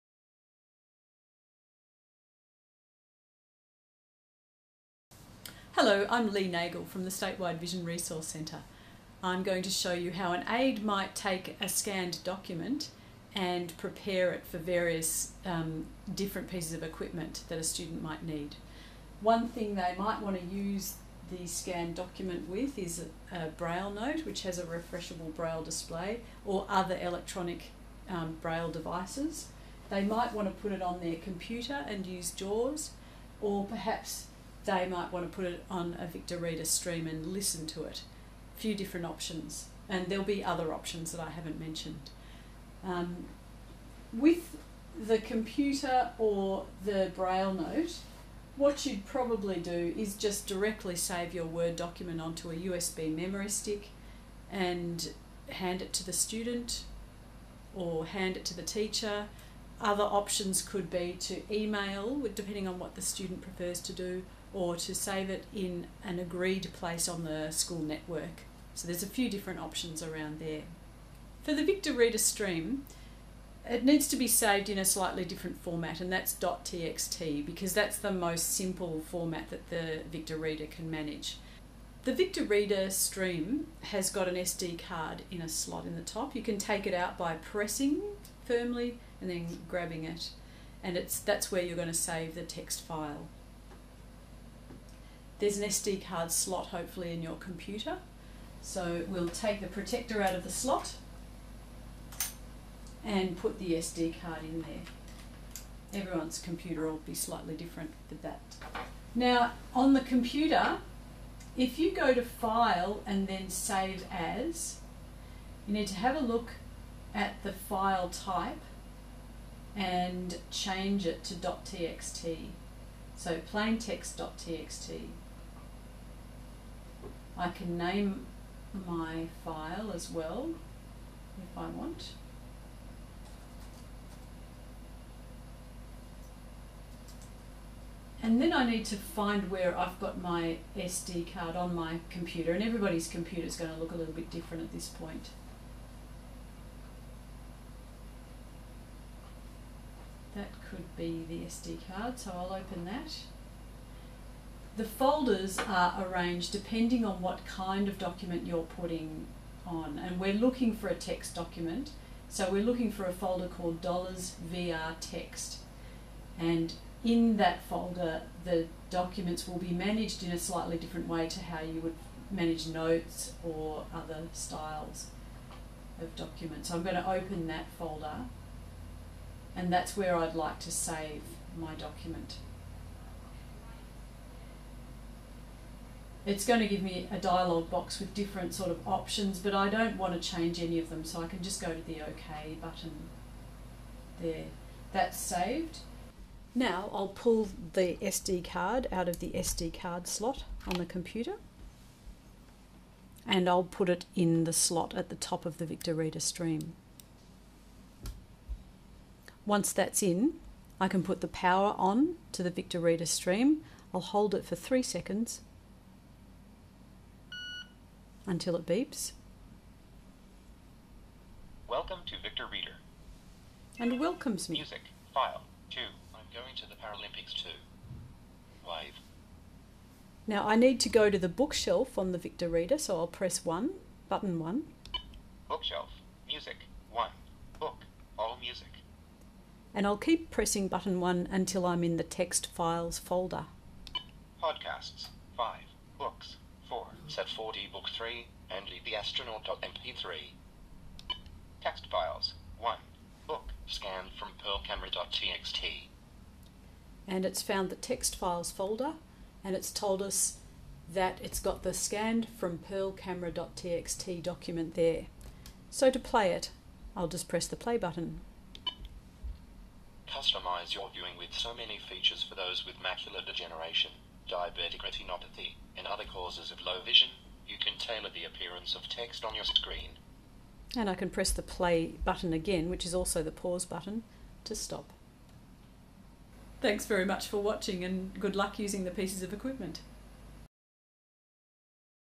Using Pearl and OpenBook - For Aides - Statewide Vision Resource Centre
Learn how you and your student can use the Pearl Camera in conjunction with OpenBook software to OCR text on-the-fly.